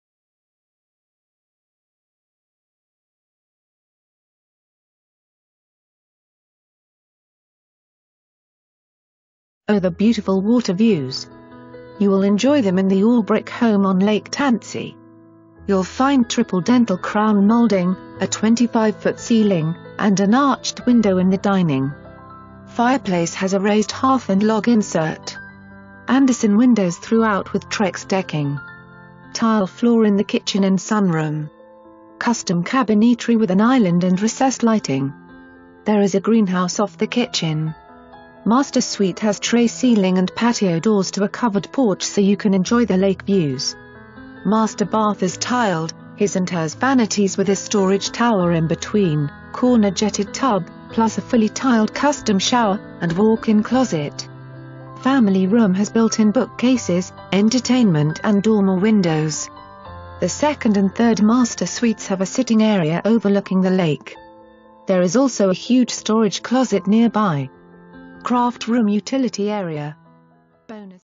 8827 Cherokee Trail
Oh the beautiful water views! You will enjoy them in the all brick home on Lake Tansi! You'll find triple dentil crown molding, a 25 foot ceiling, and an arched window in the dining. Fireplace has a raised hearth and log insert. Andersen windows throughout with trex decking. Tile floor in the kitchen and sun room. Custom cabinetry with an island and recessed lighting. There is a green house off the kitchen. Master suite has trey ceiling and patio doors to a covered porch so you can enjoy the lake views! Master bath is tiled, his and hers vanities with a storage tower in between, corner jetted tub, plus a fully tiled custom shower, and walk-in closet. Family room has built-in book cases, entertainment and dormer windows. The second AND third master suites have a sitting area overlooking the lake. There is also a huge storage closet nearby. Craft room/utility area. Bonus room has two dormer windows, two large closets with storage. Lower level has whole house storage and access doors.

Presented By:
Pam Fountain, CENTURY 21 Fountain Realty LLC